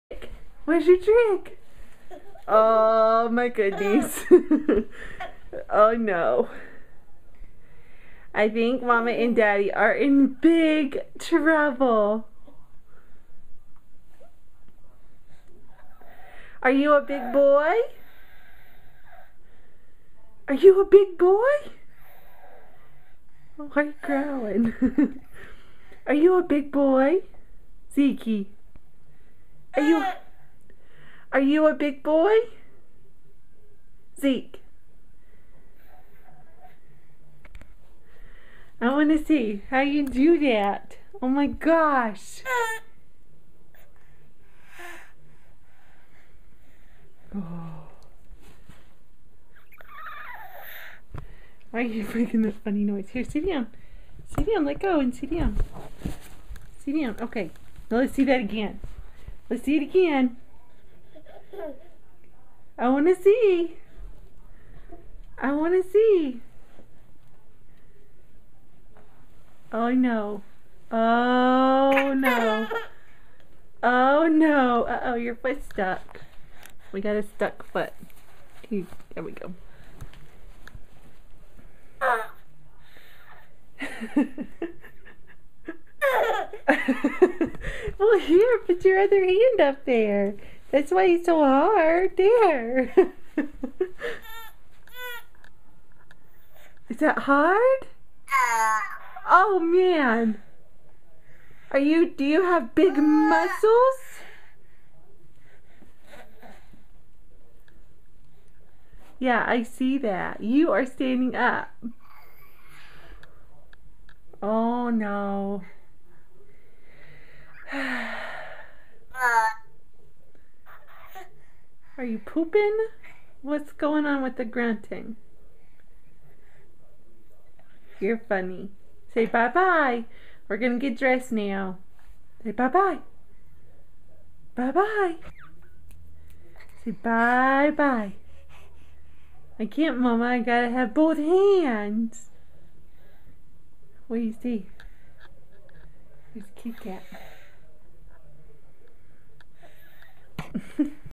Zeke Standing Up
Notice how he is grunting even after he pulls himself up? That is his new favorite sound. He grunts ALL the time!